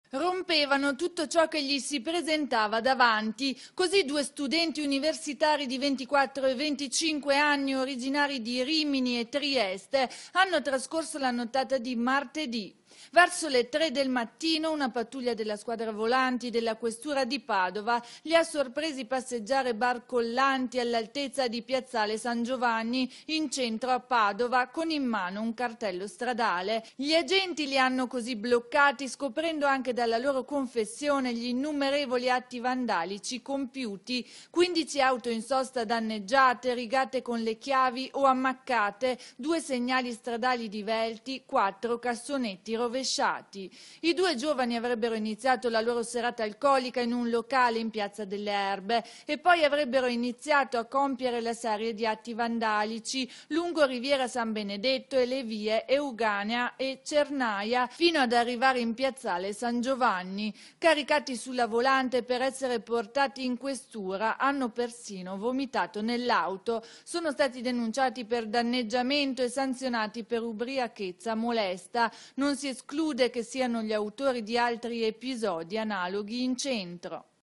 VANDALI IN CENTRO STORICO: 15 AUTO DANNEGGIATE
Cassonetti dei rifiuti rovesciati, auto in sosta strisciate con le chiavi, segnali stradali divelti. Caricati nella vola... [continua a leggere su